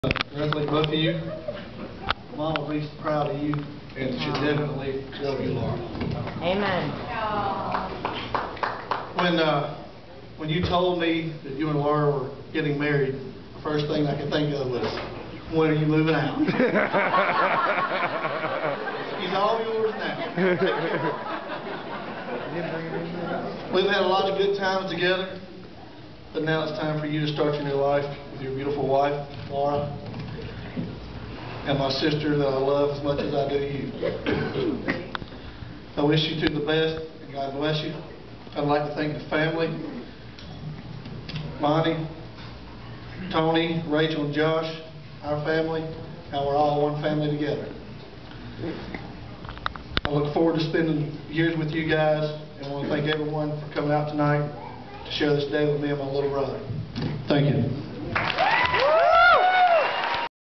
Keith Rogers
Giving his toast to his brother at his wedding